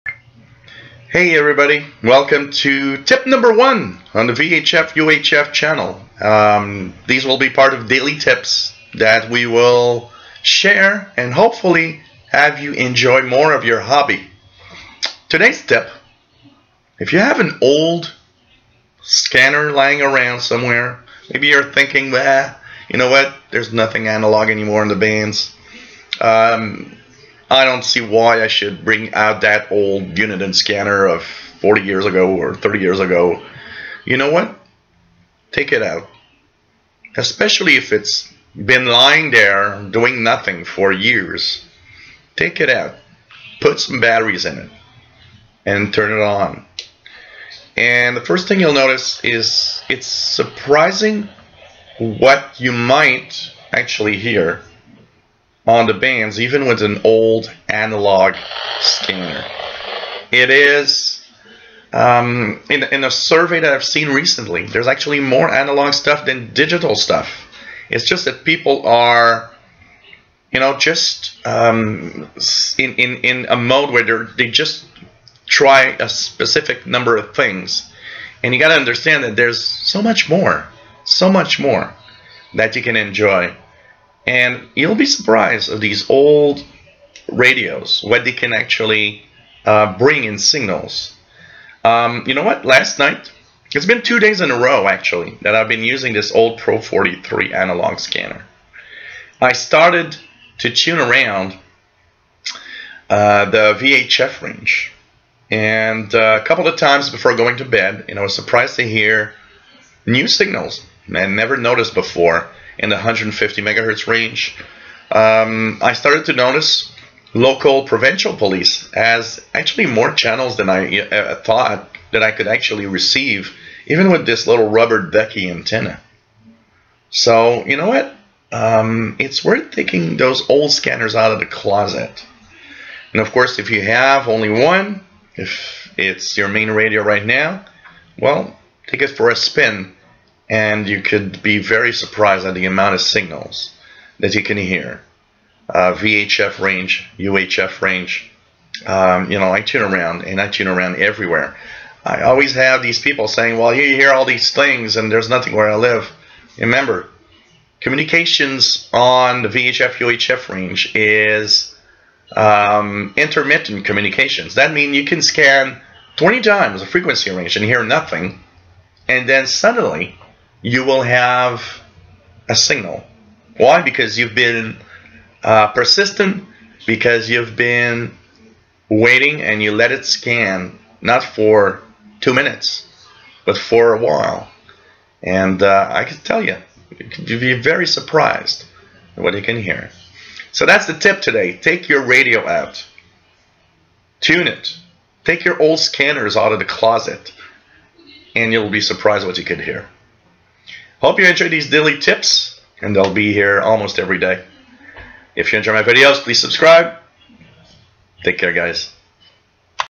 VHF UHF Tip #1 Take out the old scanner from the closet January 29th 2019
You could be surprised at the number of analog signals your old scanner can receive